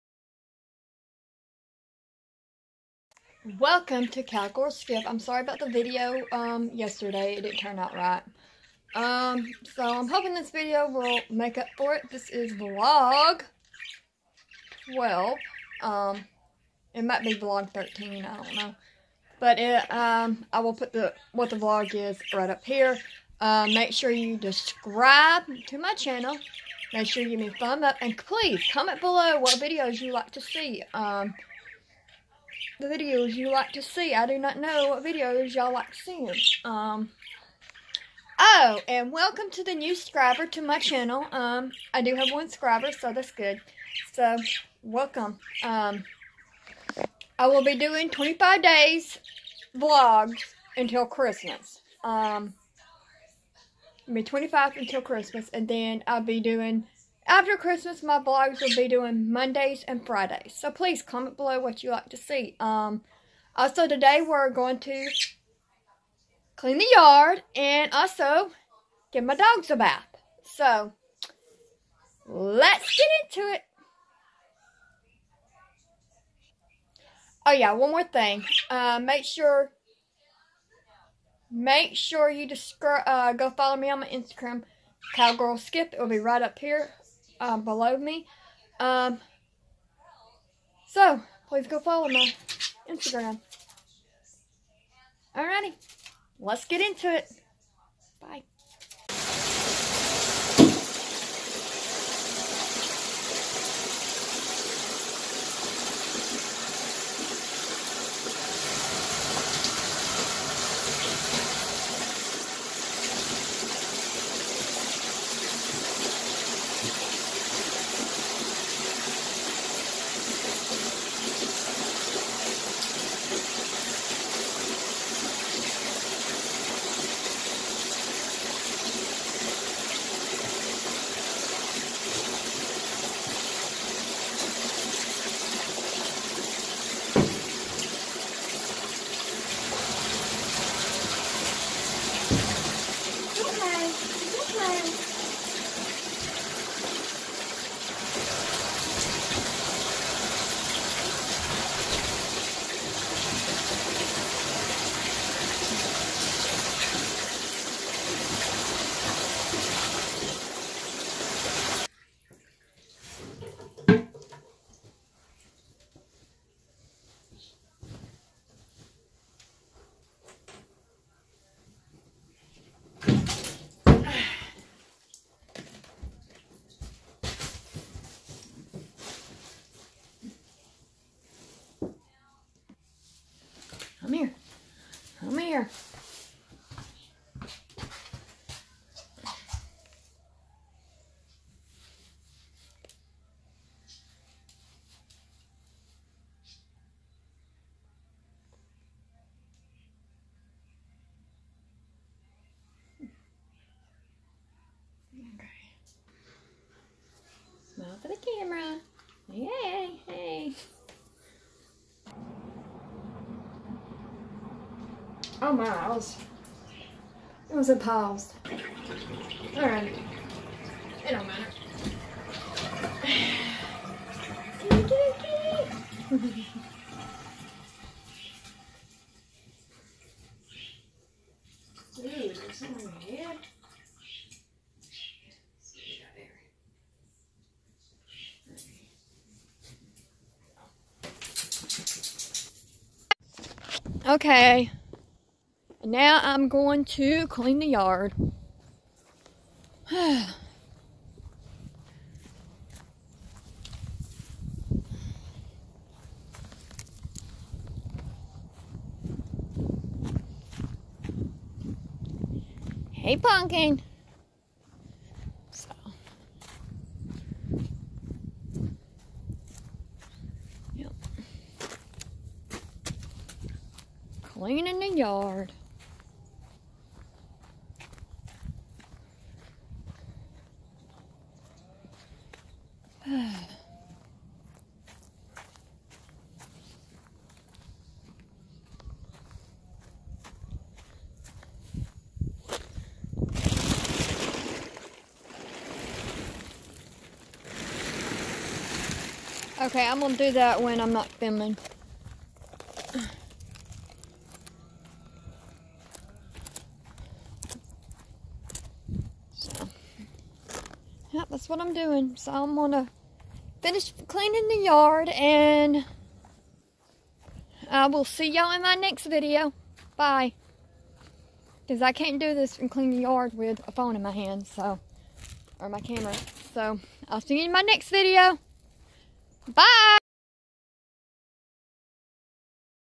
Volg Christmas 13 and giving the dogs a bath and clean the yard
Make sure you subscribe and comment below vidoe you like to see and do video 25 days till Christmas  and after Christmas I will be do video every monday and firday  and follow on my Instagram cowgirlskip